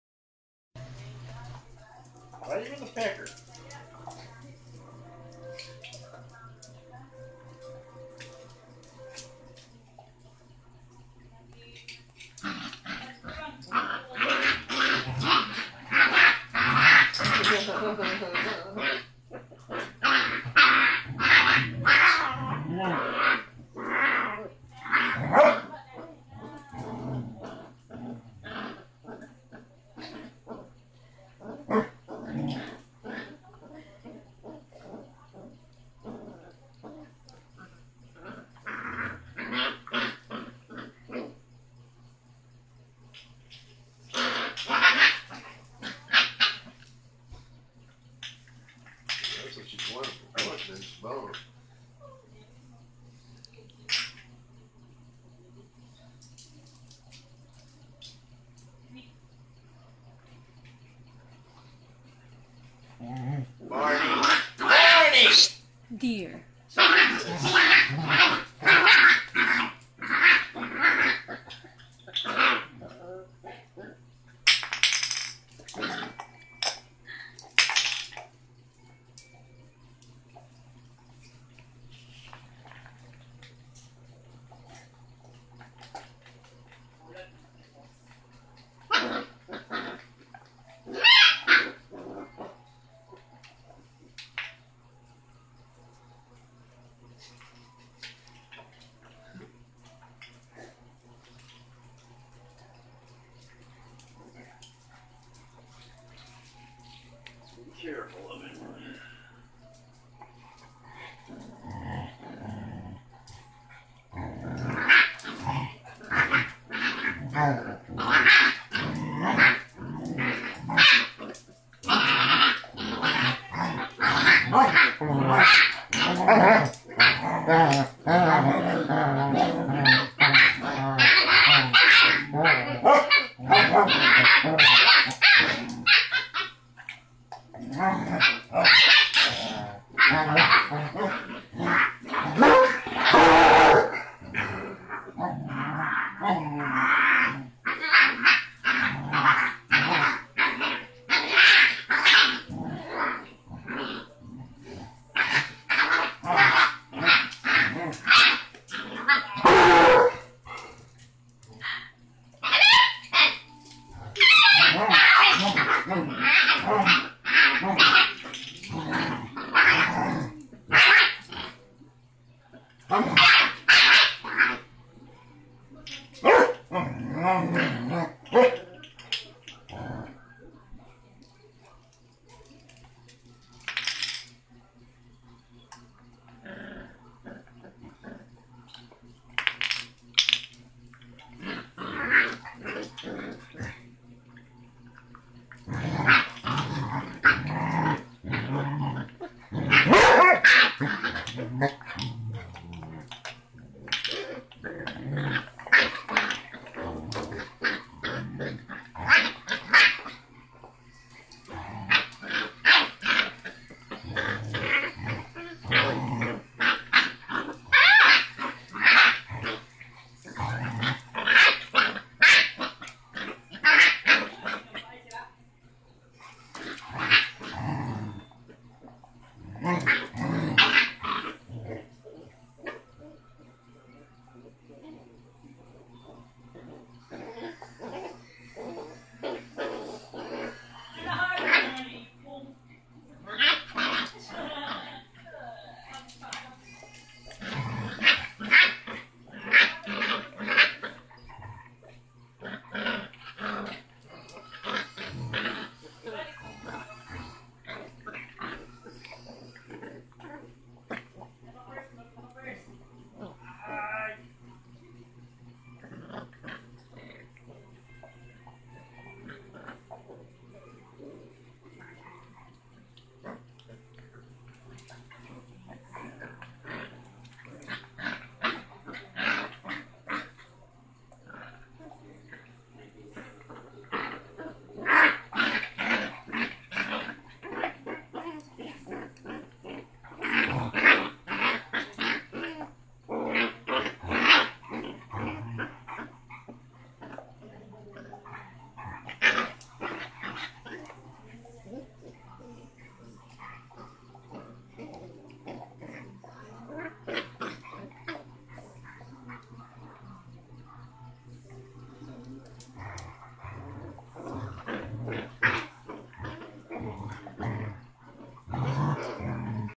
A Possessed basset hound puppy. Not Scared of Anyone
6 Week old basset hound puppy alias puppy from hell and we also call him Rocky as he beats his big brother, mom and siblings. He is the loudest than 5 of the siblings. He is the smallest but he is indeed the toughest one.

This is the 5th Litter of our Basset but, we never encounter such like these very loud, tough pup. Enjoy watching! :)